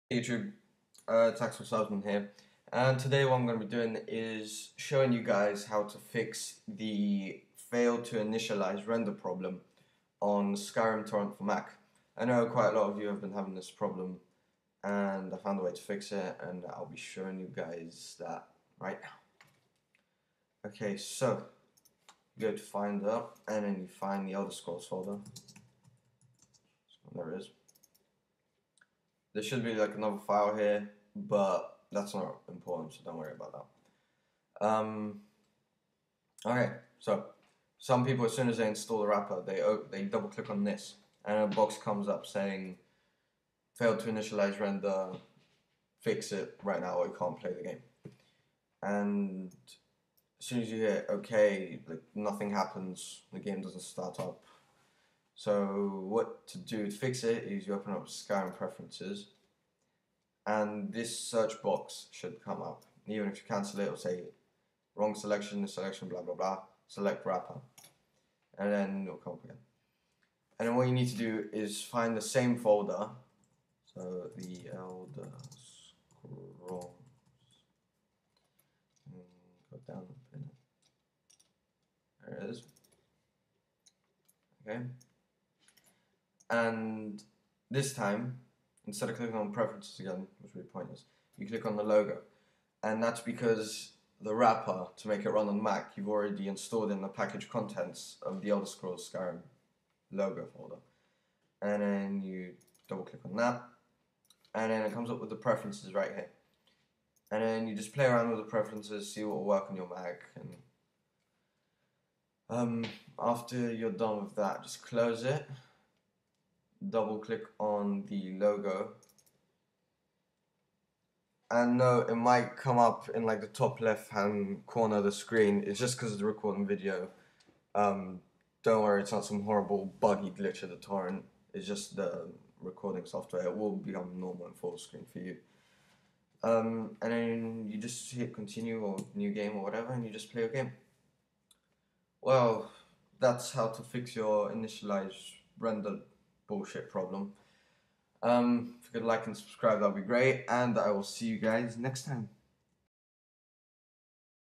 How to fix skyrim failed to initialise render mac
Hey guys this is how to fix a problem so many of you have been having, note its only a video for mac and also the game will be in full screen for you guys